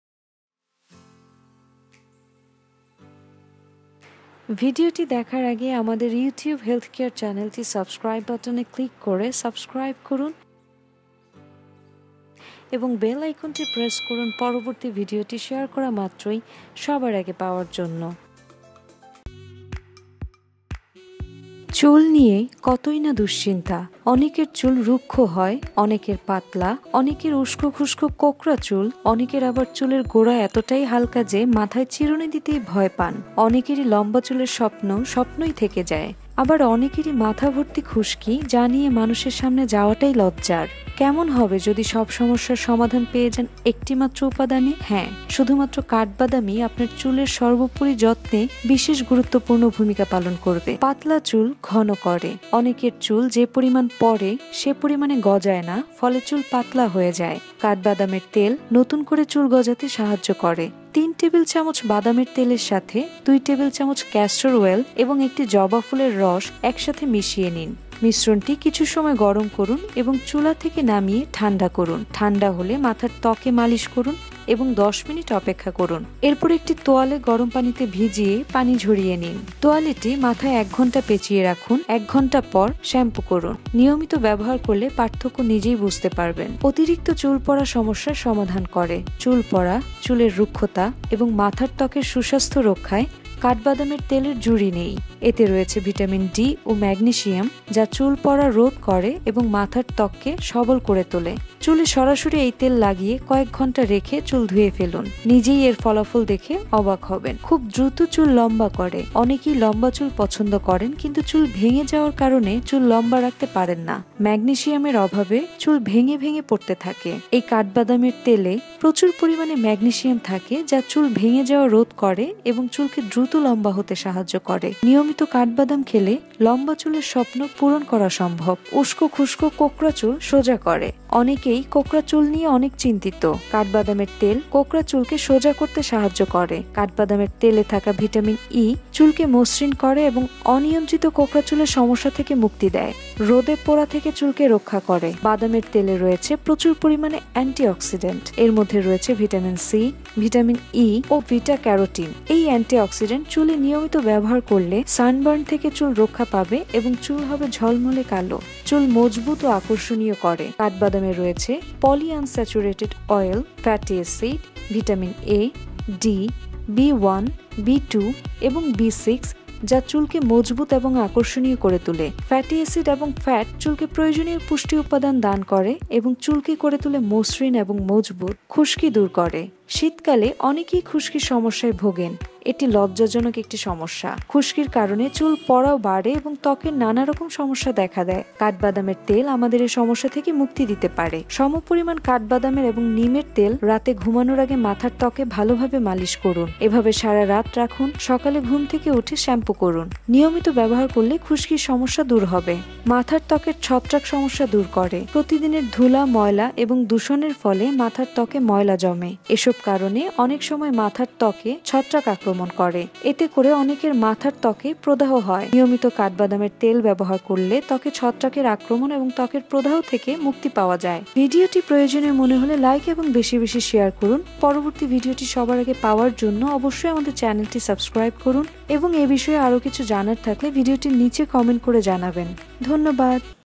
অবিশ্বাস্য হলেও সত্য! শুধুমাত্র কাঠবাদাম নিশ্চিত করবে উজ্জ্বল,ঘন,মজবুত,সোজা এবং খুশকিমুক্ত চুল!!
চুল নিয়ে কতই না দুশ্চিন্তা! অনেকের চুল রুক্ষ হয়, অনেকের পাতলা, অনেকের উস্কু-খুস্কু কোঁকড়া চুল, অনেকের আবার চুলের গোঁড়া এতটাই হালকা যে, মাথায় চিরুনী দিতেই ভয় পান। অনেকরই লম্বা চুলের স্বপ্ন স্বপ্নই থেকে যায়, আবার অনেকেরই মাথা ভর্তি খুশকি যা নিয়ে মানুষের সামনে যাওয়াটাই লজ্জার।

কেমন হবে যদি সব সমস্যার সমাধান পেয়ে যান একটি মাত্র উপাদানে। হ্যাঁ! শুধুমাত্র ‘কাঠবাদাম’-ই আপনার চুলের সর্বোপরি যত্নে বিশেষ গুরুত্বপূর্ণ ভূমিকা পালন করবে।

পাতলা চুল ঘন করেঃ
৩ টেবিল চামচ বাদামের তেলের সাথে ২ টেবিল চামচ ক্যাস্টর ওয়েল এবং ১ টি জবা ফুলের রস একসাথে মিশিয়ে নিন। মিশ্রণটি কিছু সময় গরম করুন এবং চুলা থেকে নামিয়ে ঠাণ্ডা করুন। ঠাণ্ডা হলে মাথার ত্বকে মালিশ করুন এবং ১০ মিনিট অপেক্ষা করুন। এরপর একটি তোয়ালে গরম পানিতে ভিজিয়ে পানি ঝরিয়ে নিন। তোয়ালেটি মাথায় ১ ঘণ্টা পেচিয়ে রাখুন। ১ ঘণ্টা পর শ্যাম্পু করুন। নিয়মিত ব্যবহার করলে পার্থক্য নিজেই বুঝতে পারবেন।

অতিরিক্ত চুল পড়া সমস্যার সমাধান করেঃ
চুল পড়া, চুলের রুক্ষতা এবং মাথার ত্বকের সুস্বাস্থ্য রক্ষায় কাঠবাদামের তেলের জুড়ি নেই। এতে রয়েছে ‘ভিটামিন ডি’ ও ‘ম্যাগনেসিয়াম’ যা চুল পড়া রোধ করে এবং মাথার ত্বককে সবল করে তোলে । চুলে সরাসরি এই তেল লাগিয়ে কয়েক ঘণ্টা রেখে চুল ধুয়ে ফেলুন।

খুব দ্রুত চুল লম্বা করেঃ
অনেকেই লম্বা চুল পছন্দ করেন কিন্তু চুল ভেঙ্গে যাওয়ার কারণে চুল লম্বা রাখতে পারেন না। ম্যাগনেসিয়ামের অভাবে চুল ভেঙ্গে ভেঙ্গে পড়তে থাকে। এই কাঠবাদামের তেলে প্রচুর পরিমাণে ‘ম্যাগনেসিয়াম’ থাকে যা চুল ভেঙ্গে যাওয়া রোধ করে এবং চুলকে দ্রুত লম্বা হতে সাহায্য করে।

উস্কু-খুস্কু কোঁকড়া চুল সোজা করেঃ
অনেকেই কোঁকড়া চুল নিয়ে অনেক চিন্তিত। কাঠবাদামের তেল কোঁকড়া চুলকে সোজা করতে সাহায্য করে। কাঠবাদামের তেলে থাকা ‘ভিটামিন ই’ চুলকে মসৃণ করে এবং অনিয়ন্ত্রিত কোঁকড়া চুলের সমস্যা থেকে মুক্তি দেয়।